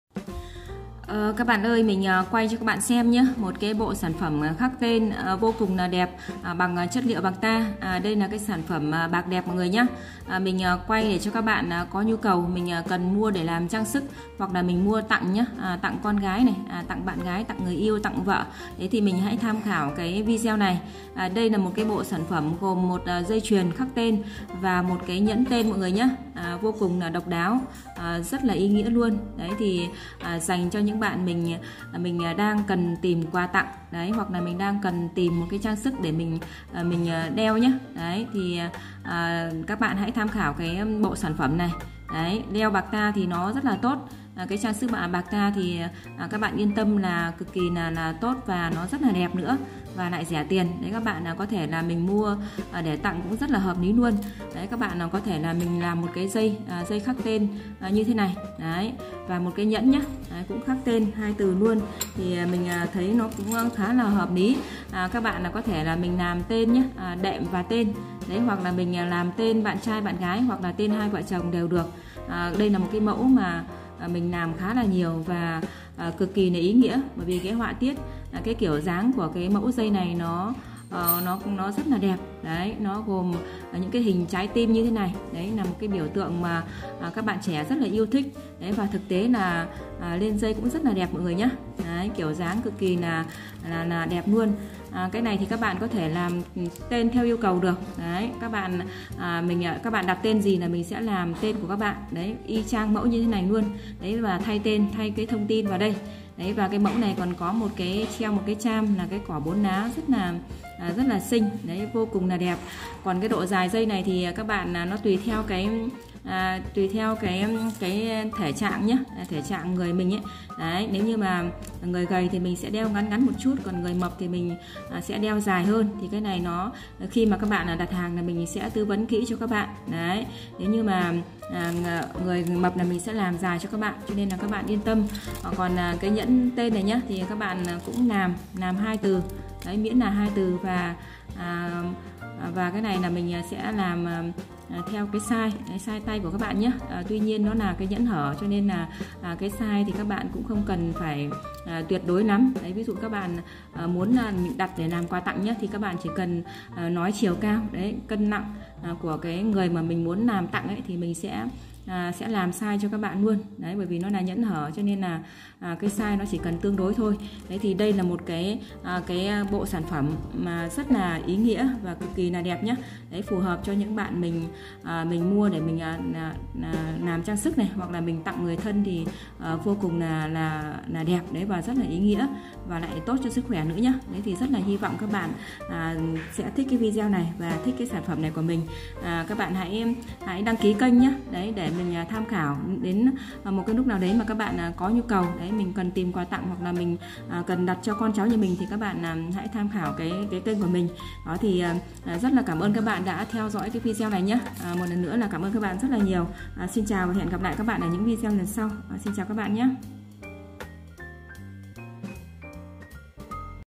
Bạc Hương: Dây chuyền bạc và nhẫn khắc tên theo yêu cầu. Nhận làm theo tên. LH: ĐT/Zlo: 0966 646 460
💥Hàng tên có rất nhiều mẫu mọi ng nhé. Bạn nào cần xem thêm mẫu thì LH zlo nhé. Tên nào cũng làm đc, thay tên và thông tin thôi ah. Đảm bảo chuẩn, đẹp, uy tín.
💥Đây là trang sức rất ý nghĩa, phù hợp làm quà tặng thay lời muốn nói, bạn nào làm tặng con gái và cháu gái cũng ok lắm. mọi ng đăng ký kênh để xem thêm mẫu nhé.